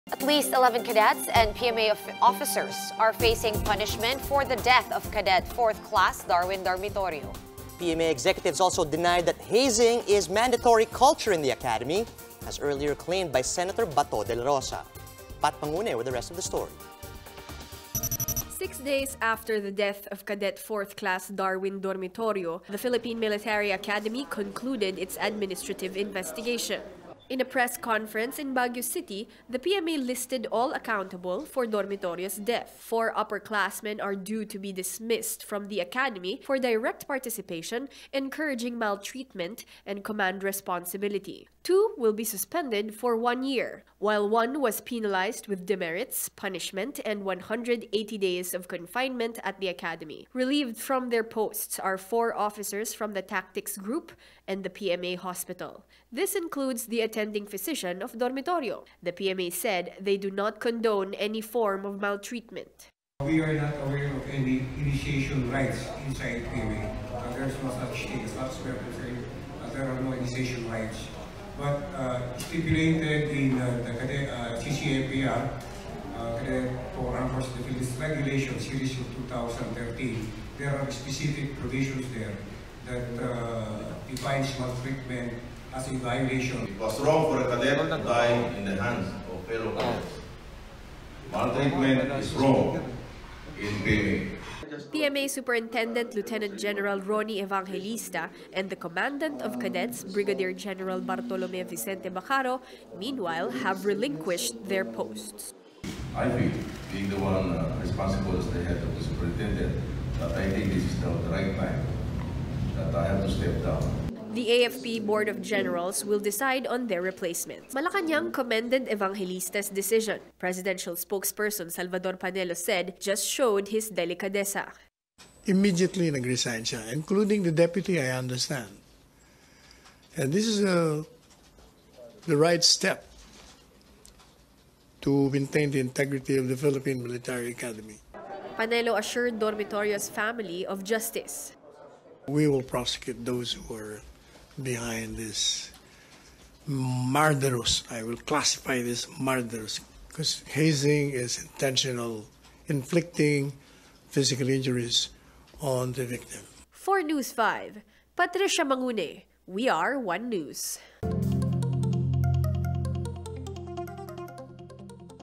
PMA concludes probe into Dormitorio's hazing death
At least 11 cadets and Philippine Military Academy officers are facing punishments for the hazing death of 20-year-old 4th class cadet Darwin Dormitorio.